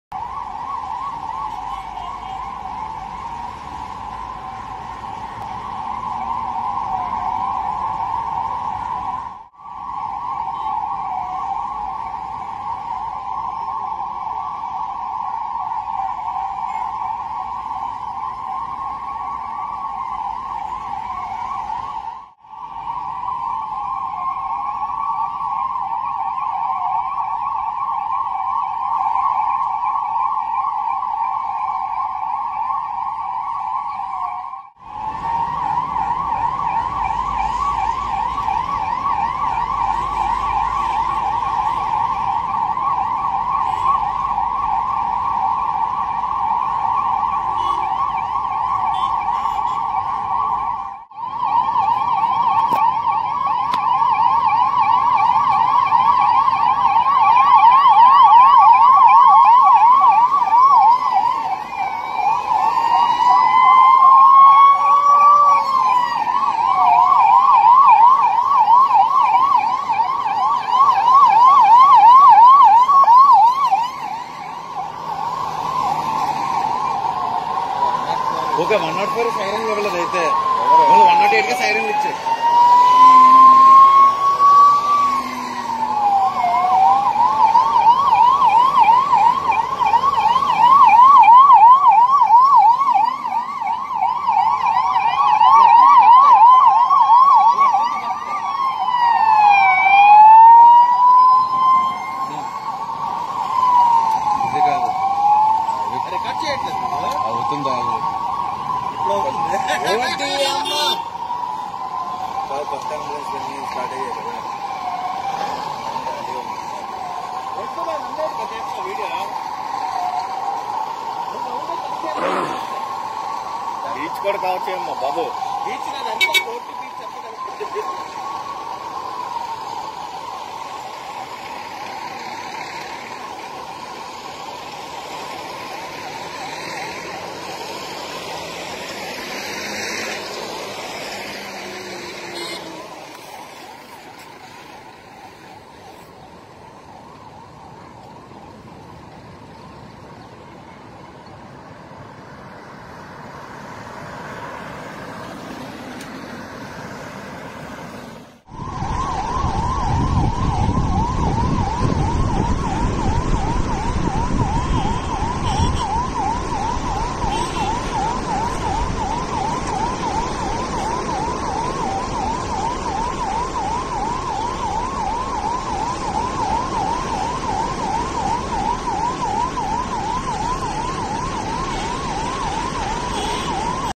108 & 104 Ambulances Rally in Vizag Beach Road || Pakka Vizag
Chief Minister Y.S. Jagan Mohan Reddy flagged-off a huge fleet of '108 ambulances' and '104 mobile medical units

Please take a look at the 108 104 Ambulances Rally in Vizag Beach Inaugurated by tourism minister Hon. Avanti Srinivasa Rao..

We are startups, so we need your valuable feedback.

More interesting and beautiful vizag videos are on the way, keep following us.